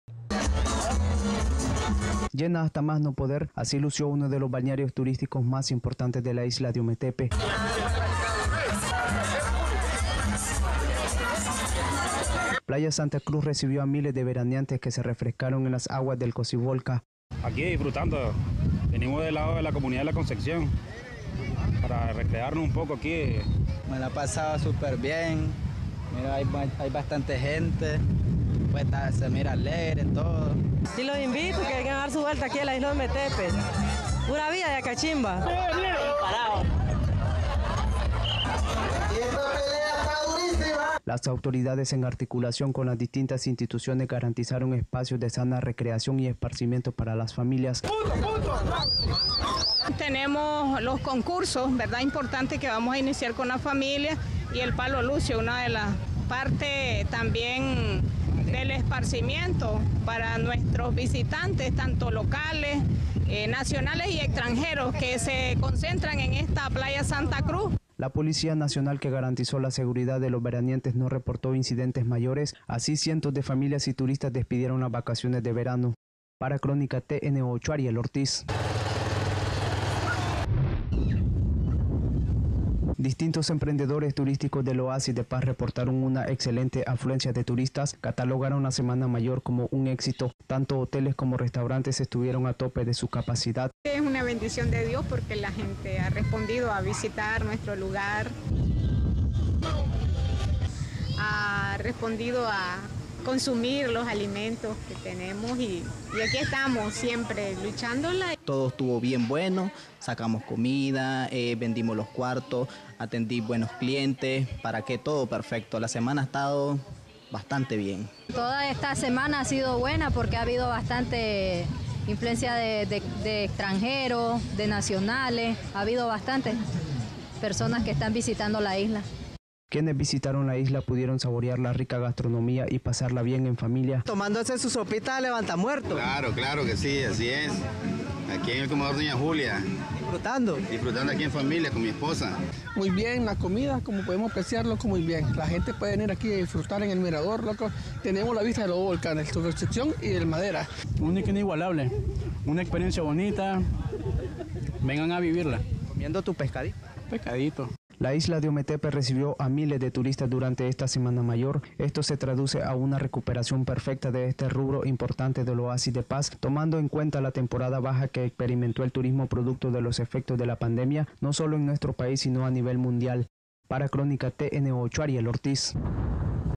Veraneantes disfrutan de las playas de la Isla de Ometepe - Nicaragua
Llena hasta más no poder, así lució uno de los balnearios turísticos más importantes de la Isla de Ometepe. Les hablamos de playa Santa Cruz; que recibió este sábado de gloria a miles de turistas, nacionales, locales y extranjeros. La enorme cantidad de vehículos no permitía el tráfico de personas sobre la carretera principal; por lo que fue necesario que efectivos de la Policía Nacional intervinieran para regular el tráfico.

Al ritmo de música, al calor del sol y en familia; así disfrutaron los veraneantes de las aguas de esta bella playa de arena blanca en la que miles se desbordaron para ponerse en contacto directo con la naturaleza y olvidarse de la rutina.

«Muy bonito todo. Primera vez que vengo a la Isla de Ometepe y ando con mi mama y con toda mi familia. Andamos disfrutando, venimos de Río San Juan» dijo Michael García.

Veraneantes disfrutan de actividades recreativas
La alcaldía de Altagracia compartió con los asistentes, una tarde recreativa y deportiva en la que hubo boxeo, baile y palo lucio. Los ganadores recibieron premios en efectivo y souvenires. Según la alcaldesa Aurora Álvarez, se ejecutó todo un plan de fue desde limpieza de playas, hasta actividades de esparcimiento para las familias y turistas.

«Un día muy hermoso para las familias en el disfrute y la recreación en las diferentes playas de nuestra Isla de Ometepe. Uno de los temas más importante es la seguridad; también la limpieza de todas las playas y el tema de la recreación. Para garantizar todo esto se realizó un trabajo articulado entre todas las instituciones de nuestro Gobierno», expresó la funcionaria pública.

Este domingo se espera que nuevamente miles de turistas despidan las vacaciones visitando todos los balnearios de este destino turístico.